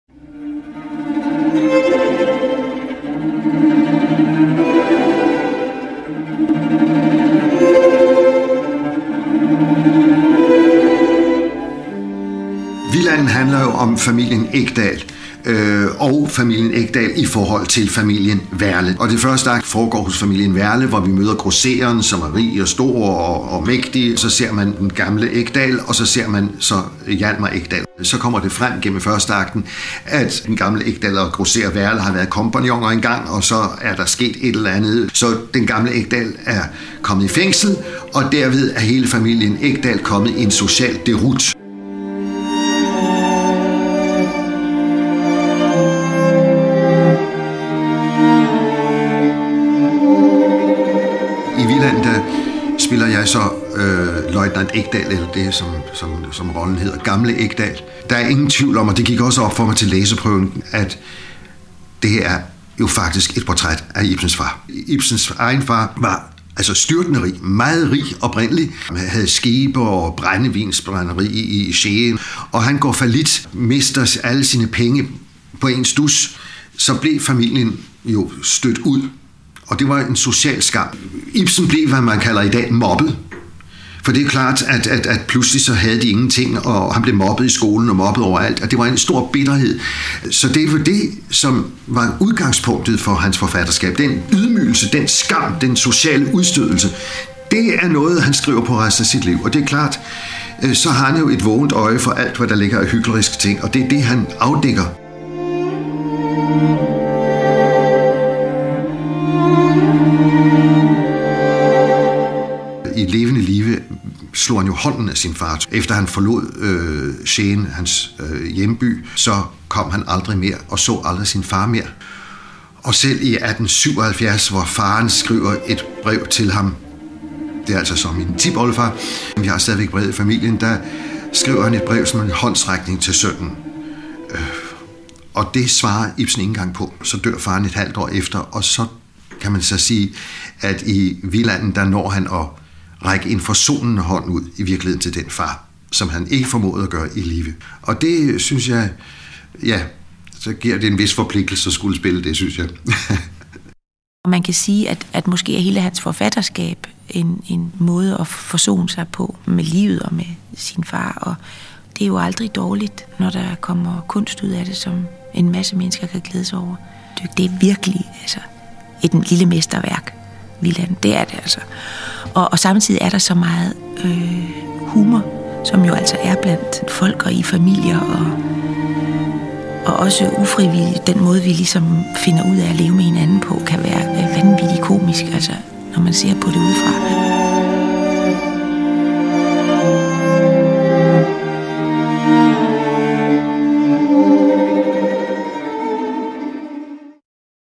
Vildanden
**Hør skuespiller Joen Bille fortælle om hans tipoldefar Henrik Ibsens liv og forfatterskab